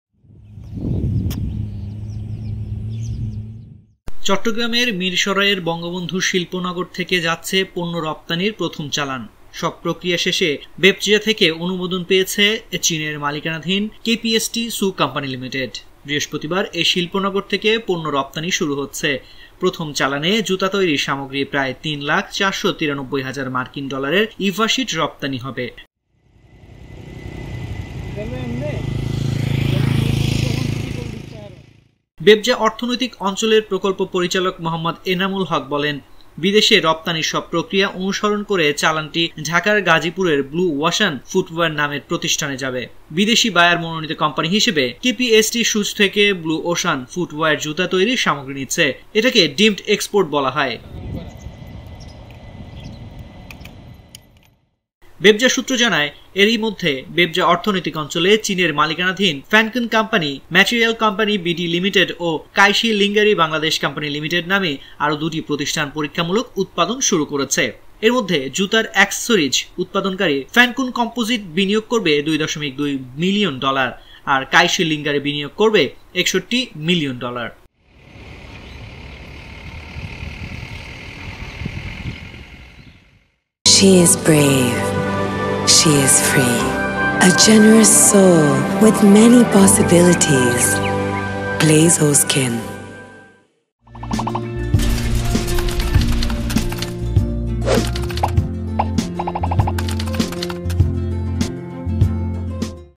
বঙ্গবন্ধু শিল্পনগরে উৎপাদিত রপ্তানি পণ্যের প্রথম চালান যাচ্ছে আজ || Jago News
বঙ্গবন্ধু শিল্পনগরে উৎপাদিত রপ্তানি পণ্যের প্রথম চালান যাচ্ছে আজ